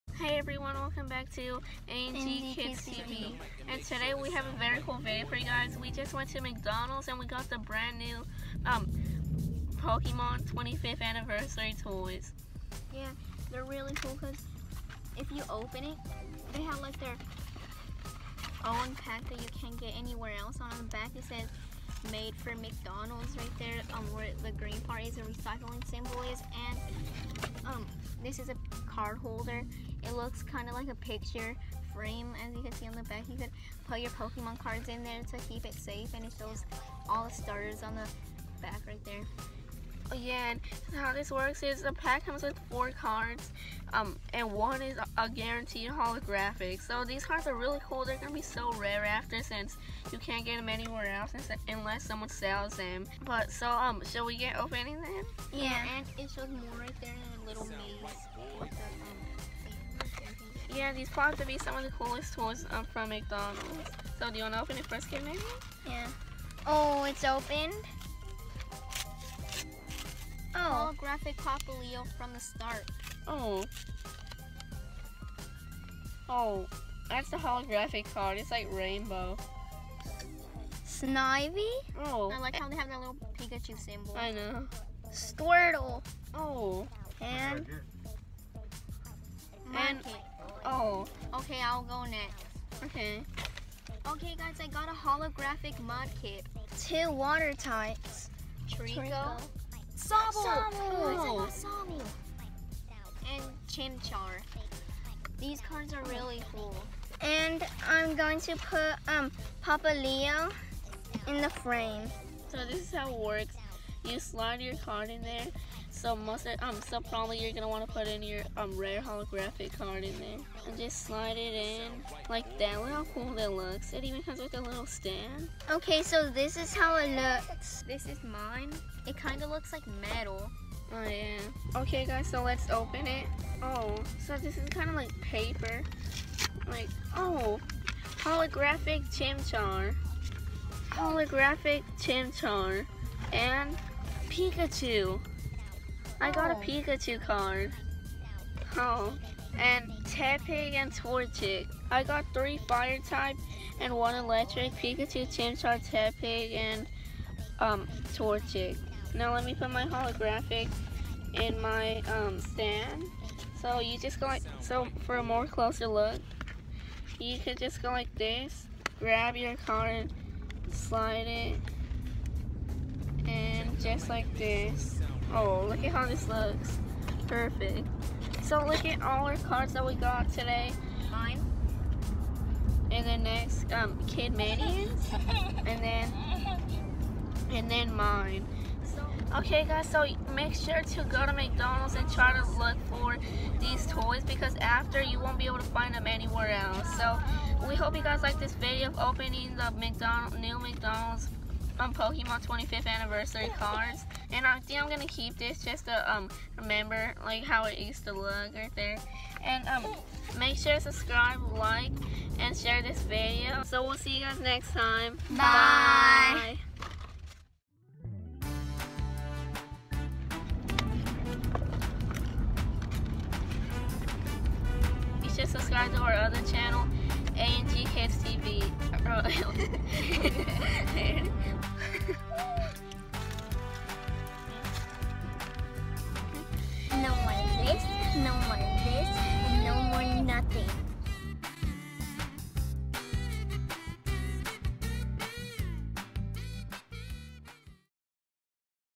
McDonald's 25th Anniversary Pokemon Cards Opening!
In this Video Will will be Opening the New Pokemon 25th Anniversary Pokemon Cards, Only at McDonald's! These cards are Very Special Because You can only get them for a Limited Time, and you can't get them Anywhere else! Have you got the New Pokemon Cards?